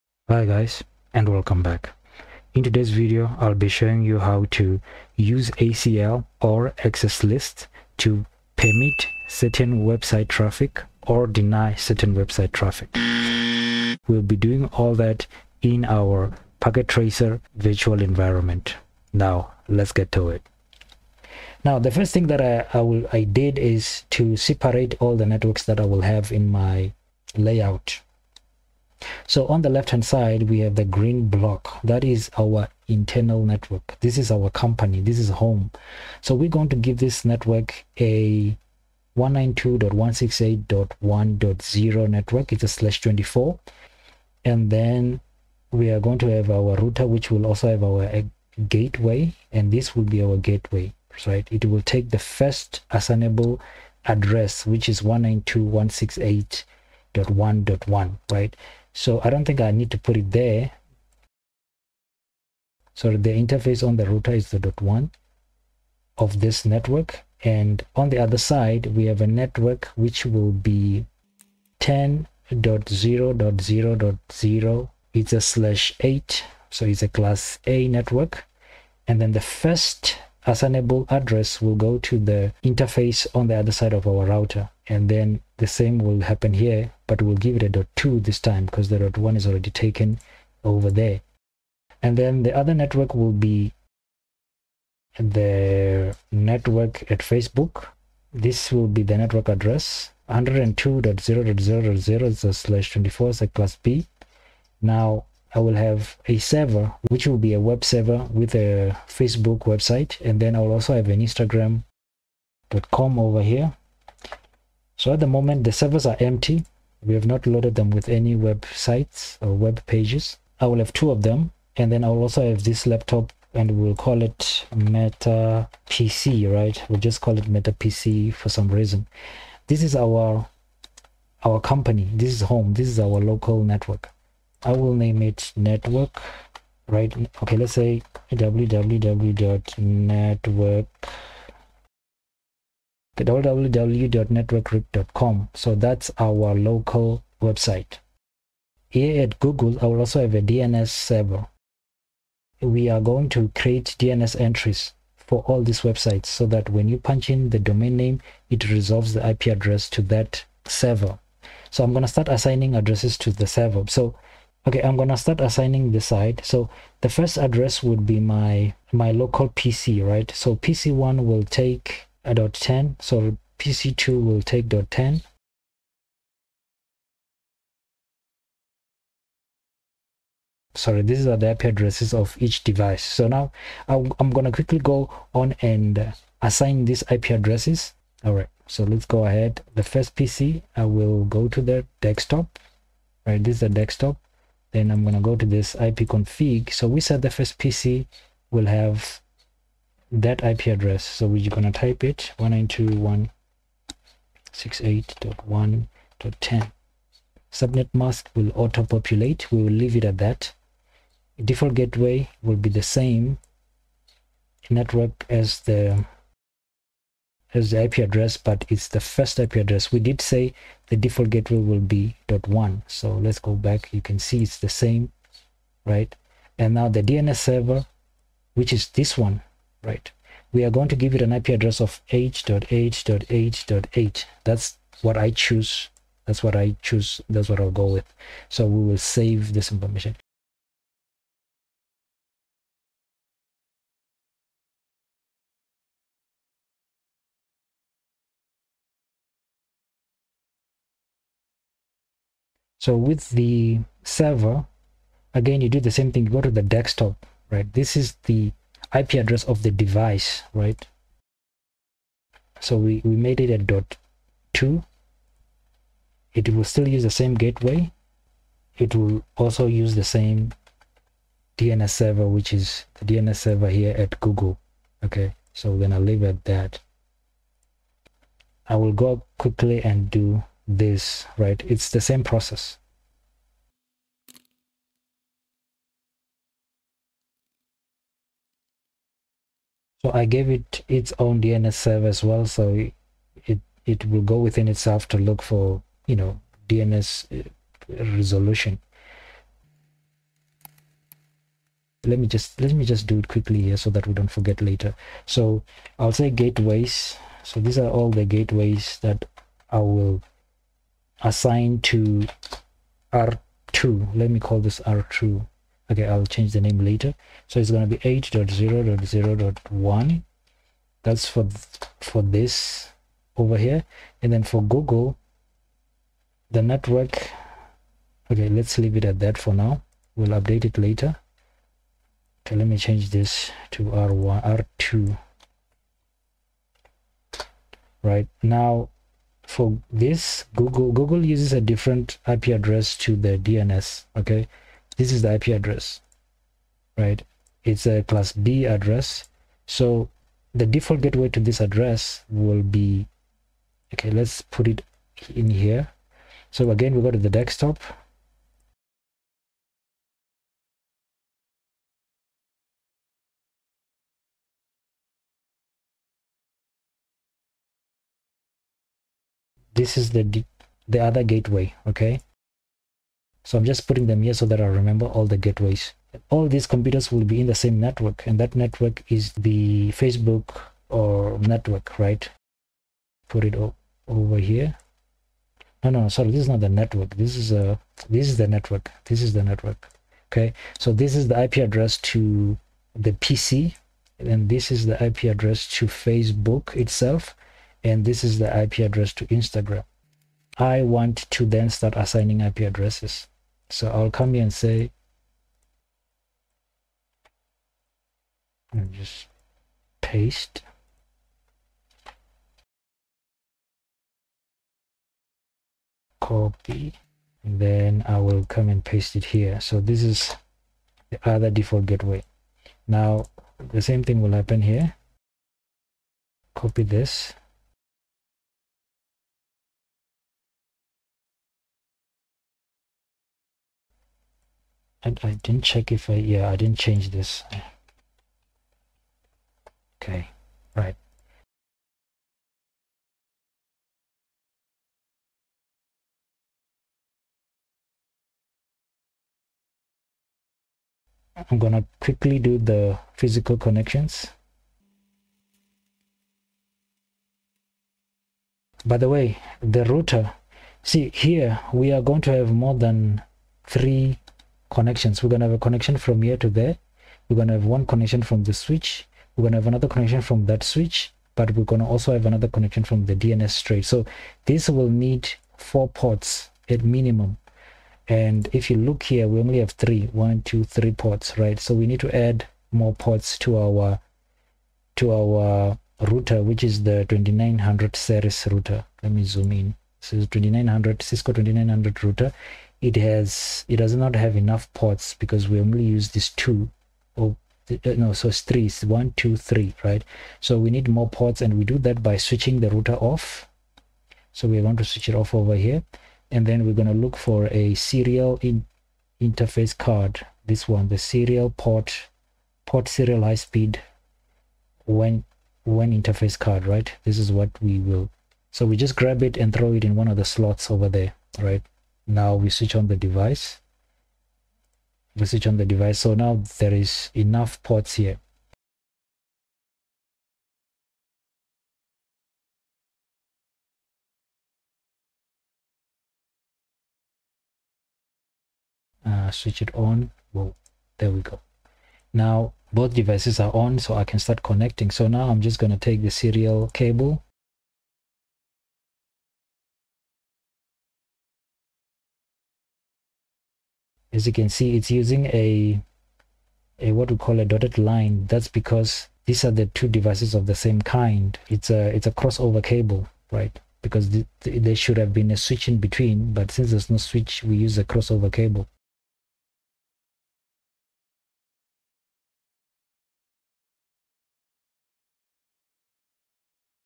Access Control List (ACL) Part 1: Configure IP Address | Beginners
Block Facebook or Google using ACL on Cisco Packet Tracer virtual environment
Part 1:  We setup all devices and configure IP address on web server.

Deny and Permit
Internet access
Websites
Web server
Facebook, Instagram, Google
port 80
Access-group
Access-list
Webserver